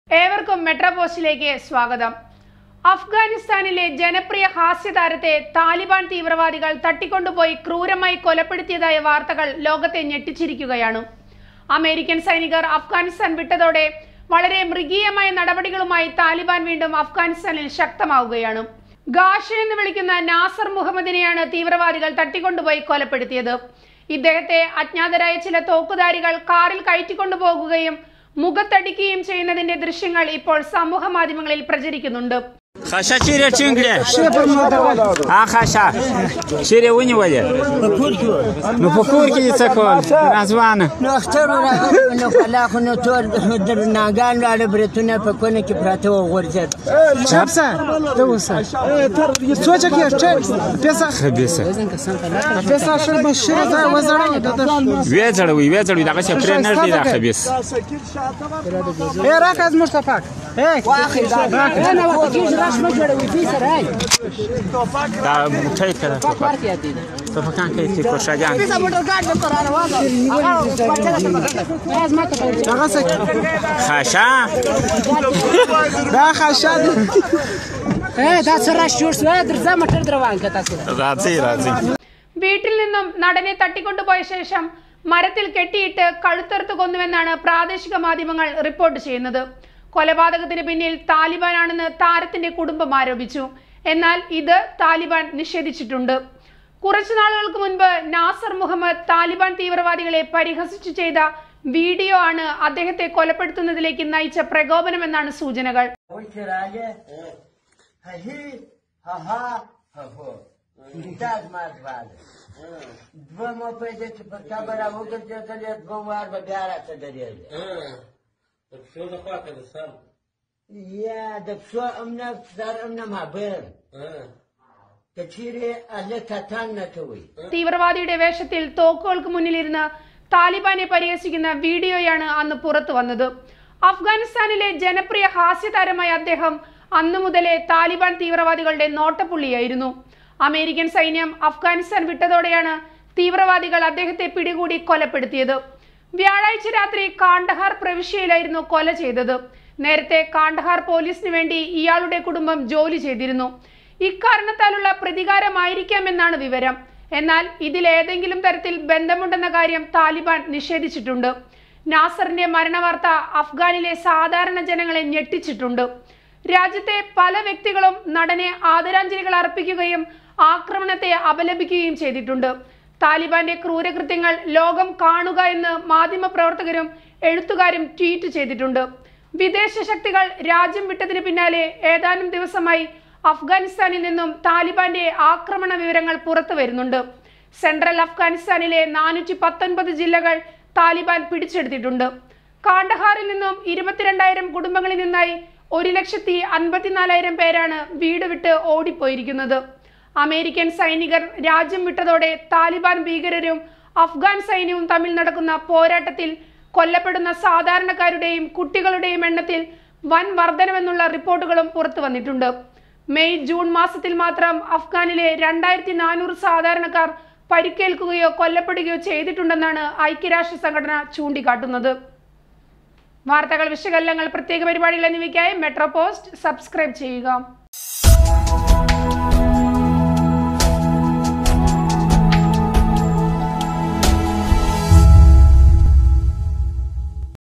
അഫ്ഗാൻ ഹാസ്യ താരത്തെ കഴുത്തറുത്തു കൊല്ലാൻ താലിബാനെ പ്രകോപിപ്പിച്ച വീഡിയോ ഇതാണ് | METRO POST
Afghan comedian's brutal murder sends shock waves around world; Taliban denies killing him